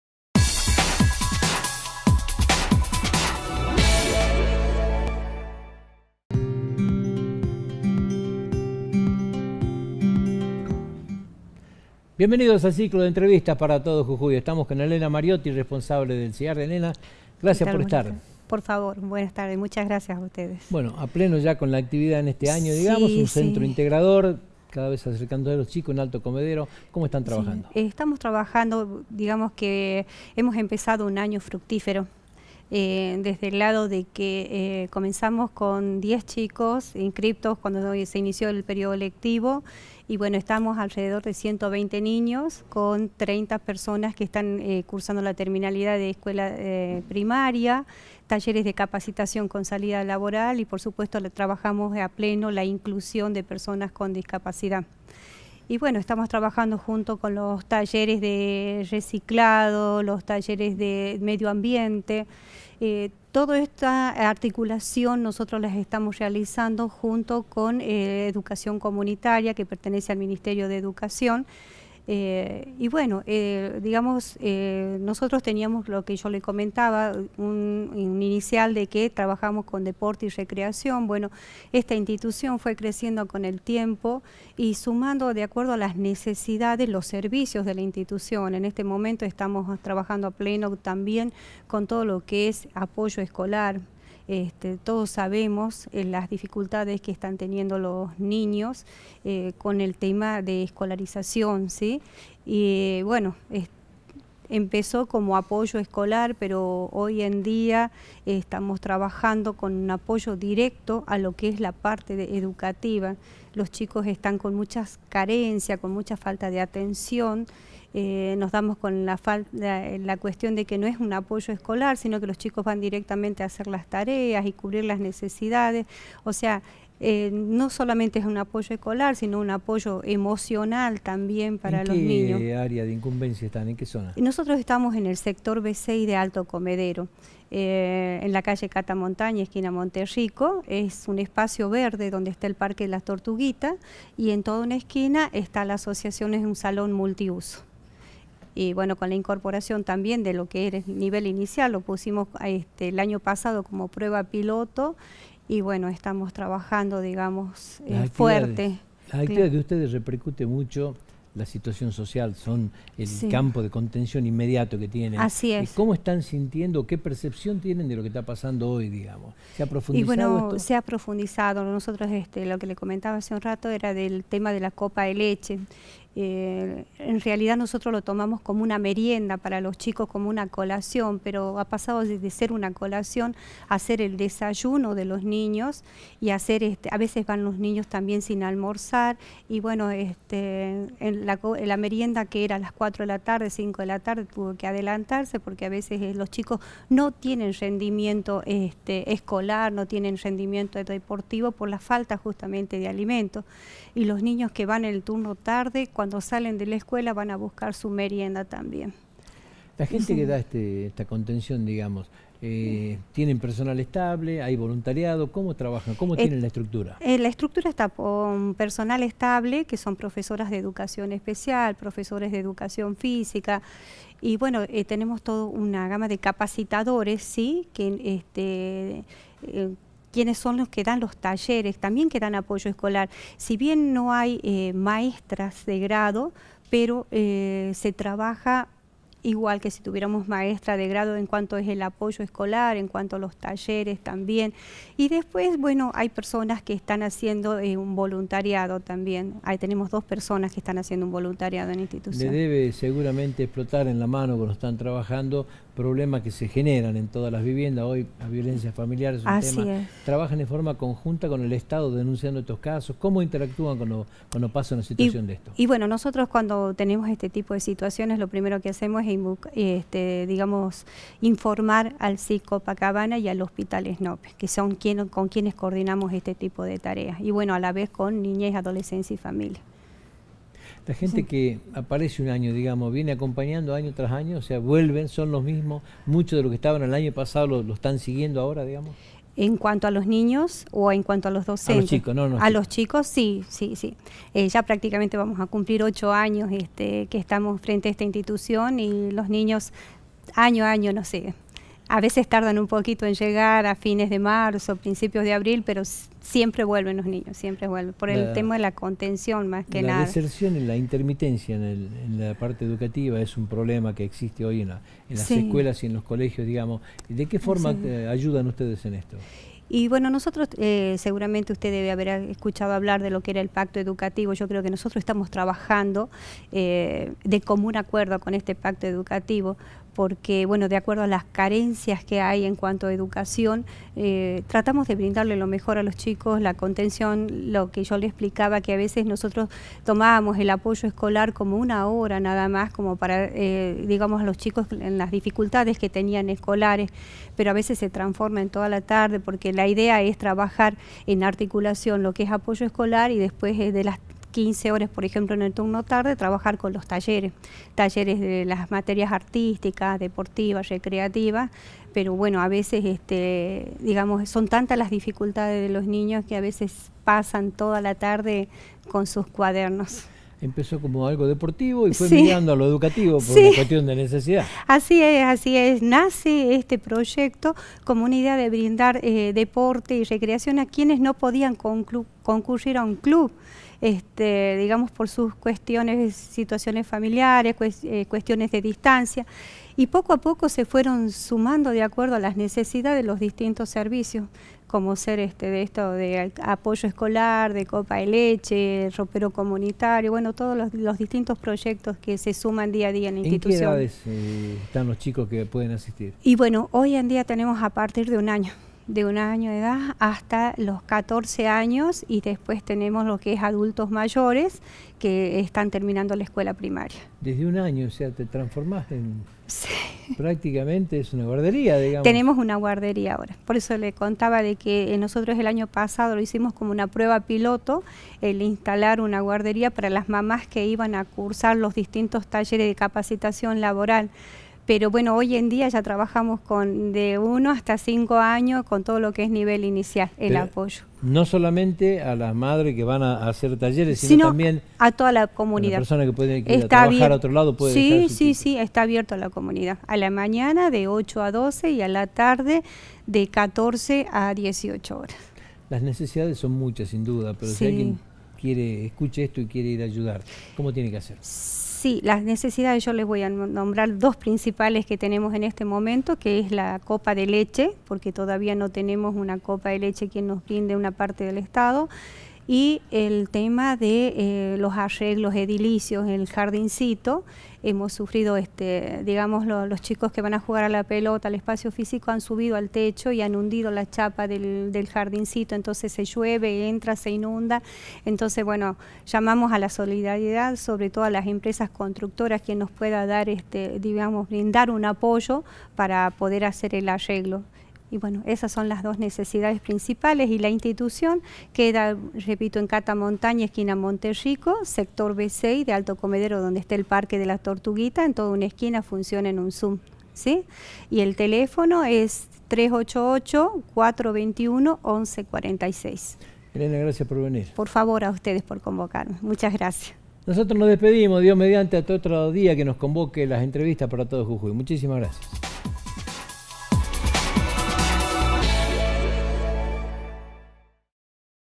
ELENA MARIOTI
Elena Mariotti: “Brindamos contención a niños y jóvenes de Alto Comedero”
La presidente del CIARD comentó que en la organización que nuclea contienen a niños y jóvenes del barrio Alto Comedero y solicitó el apoyo de la comunidad para hacer un mejor trabajo para toda la sociedad.